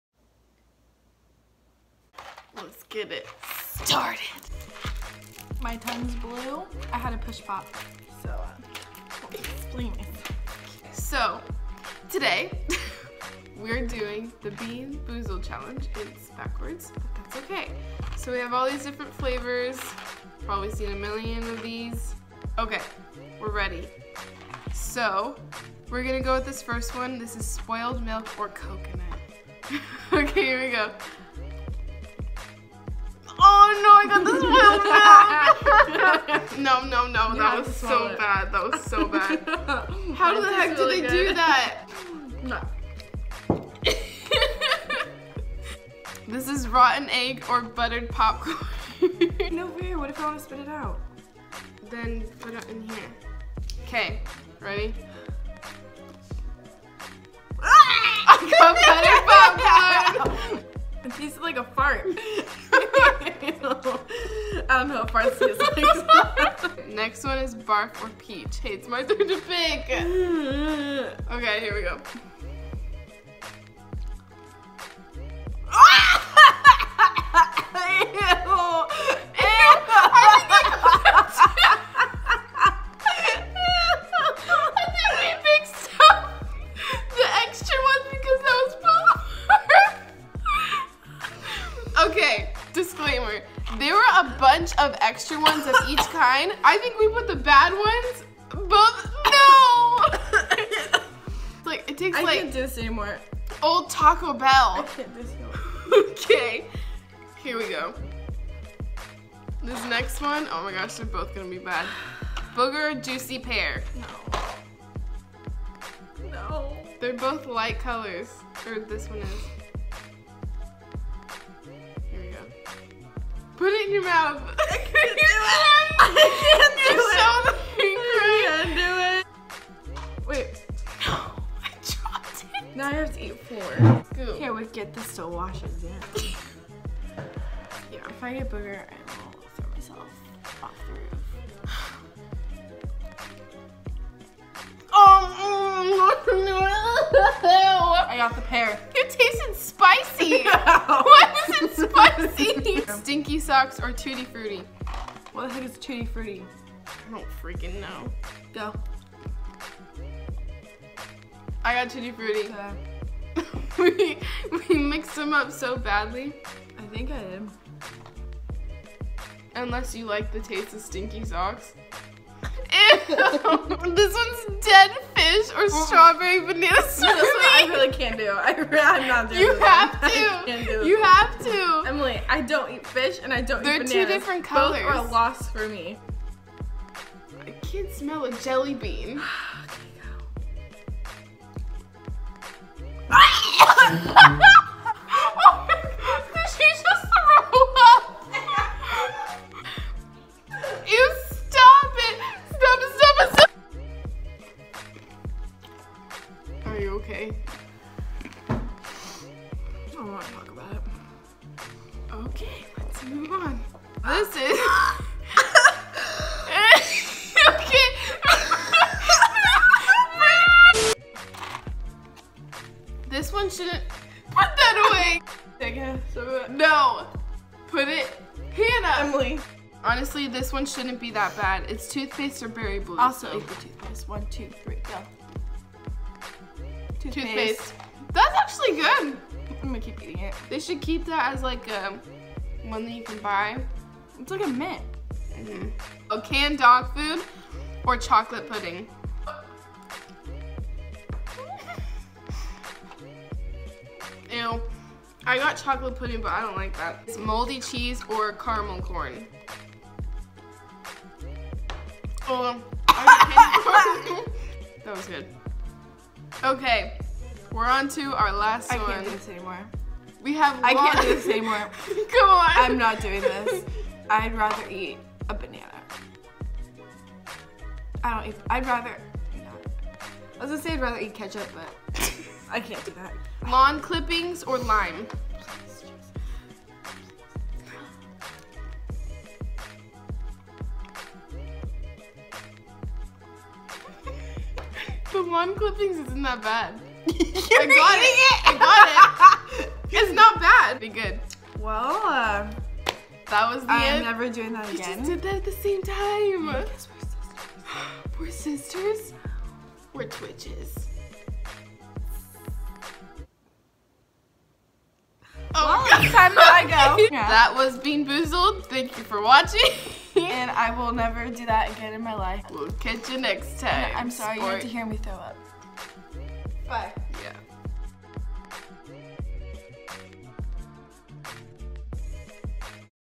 HILARIOUS BEAN BOOZLED CHALLENGE (actually went wrong *CHECK DESCRIPTION*)
My sister and I try the "Bean Boozled Challenge"! It got real messed up, these things are rigged, man. (Sorry, some of the audio cut out!)

☆ Social Media ☆